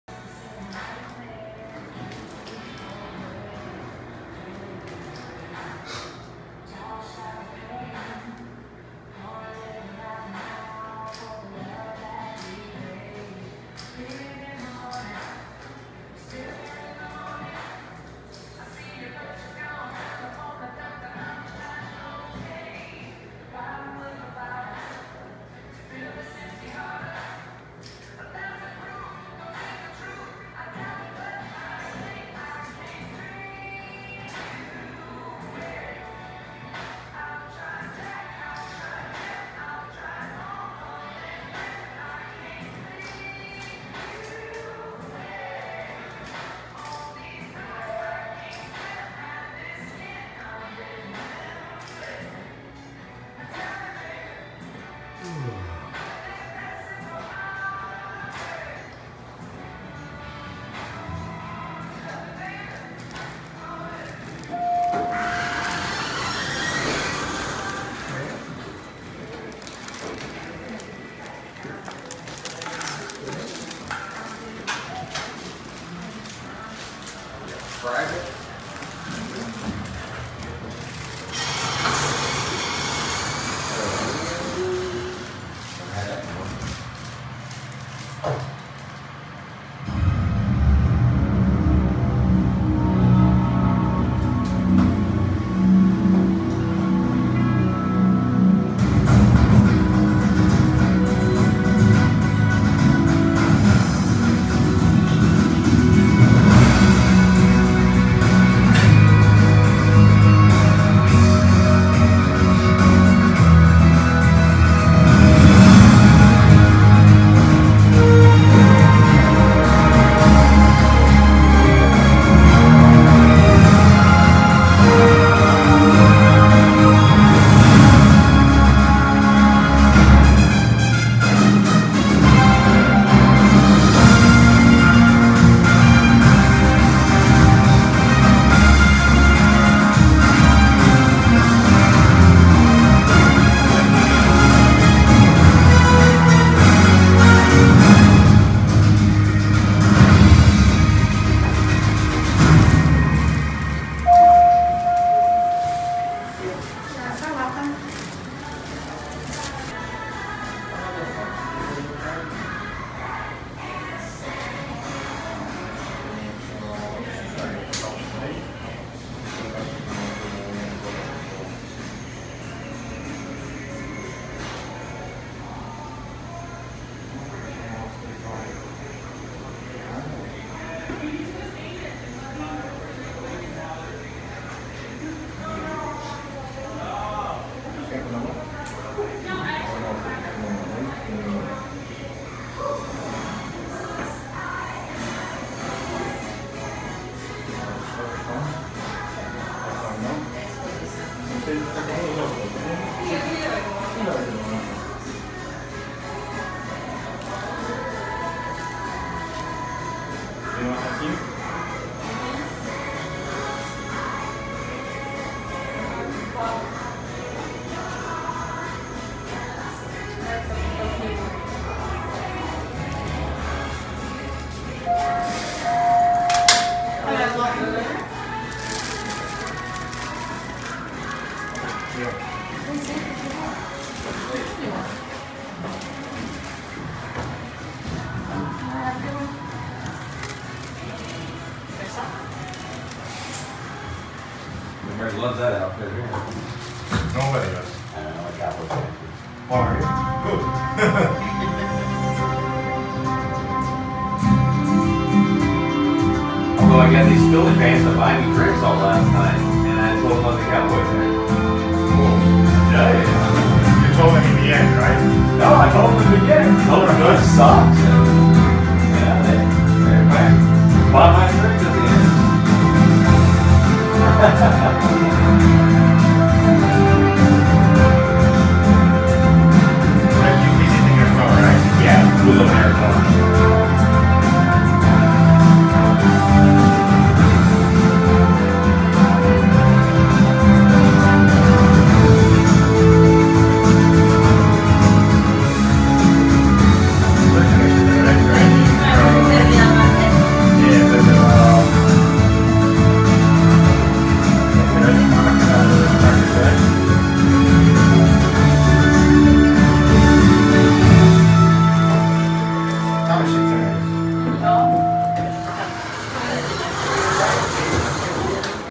4K 60fps: OTIS Traction Elevators At One Liberty Place Observation Deck - Downtown Philadelphia
NOTE: 4K 60fps video playback may not be supported on some devices. The link to the 30fps version of this video is here in the description for the devices that can't do 60fps video playback
Brand: OTIS
Type: Traction
Capacity: 7000 lbs.
Fixtures: (none)
Motor: Unknown
Total Floors: 59 (LL2, LL1, G, 2-57) (LL2 & LL1 are restricted)